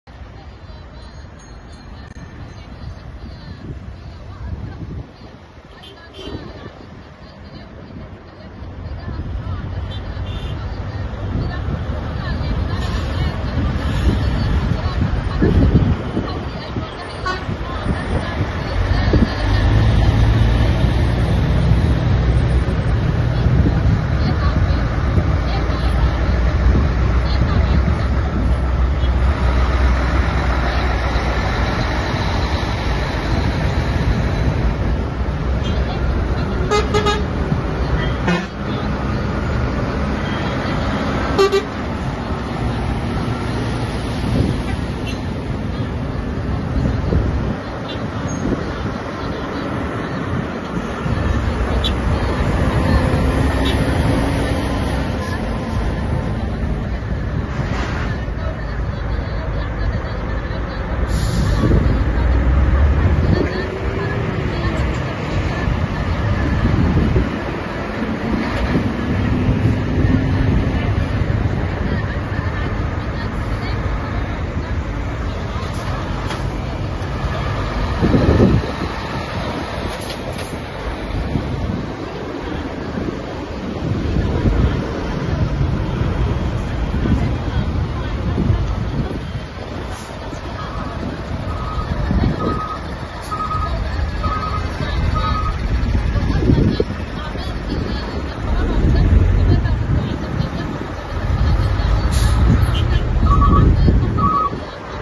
مسافة السكة لأهالينا في فلسطين.. أكبر قافلة مساعدات إنسانية يقودها التحالف الوطني
مسافة السكة لأهالينا في فلسطين.. أكبر قافلة مساعدات إنسانية يقودها التحالف الوطني للعمل الأهلي التنموي
مئات السيارات المحملة بألاف الأطنان من المواد الغذائية والمساعدات الطبية في طريقها لفلسطين